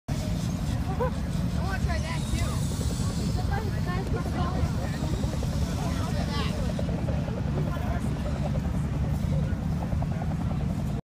Bomb Robot Recovers Snowcone.
An Explosive Ordinance Disposal robot picks up a snow cone.